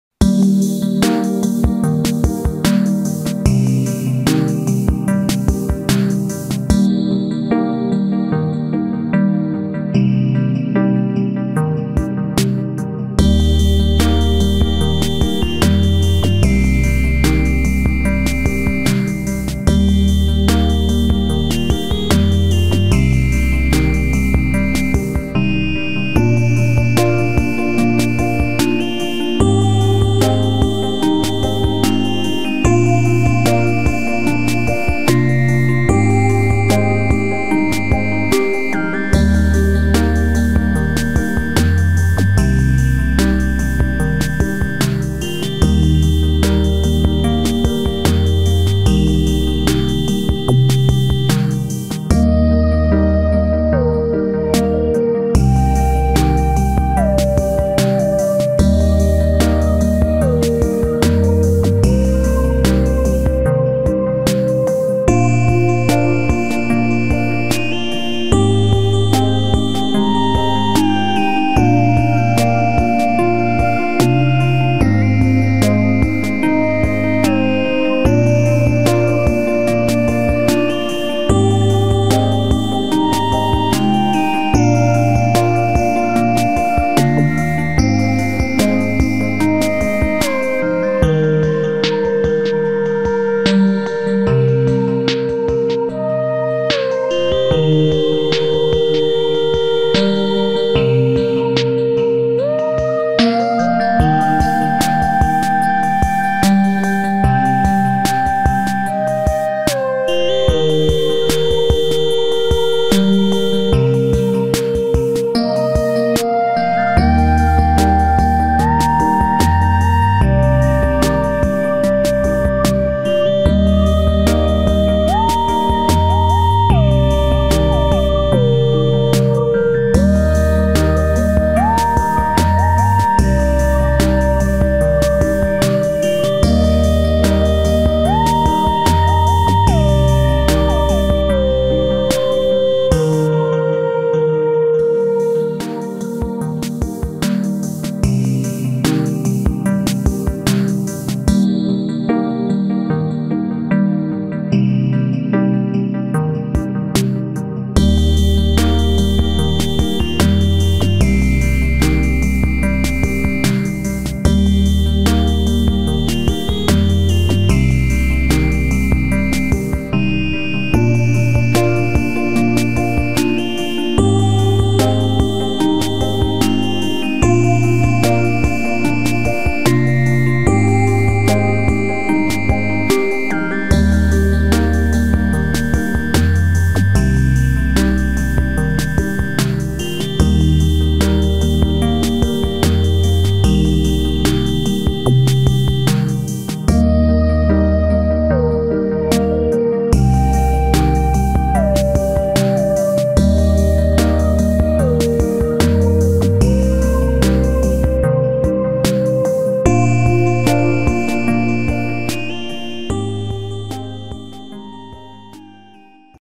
Quatis no sitio Ganapati
Hoje de manhã, um grupo de quatis veio até perto de nossa casa, para comer mangas. Esse bando tem várias fêmeas adultas e muitos filhotes. É uma grande alegria ver esses bichinhos se alimentando!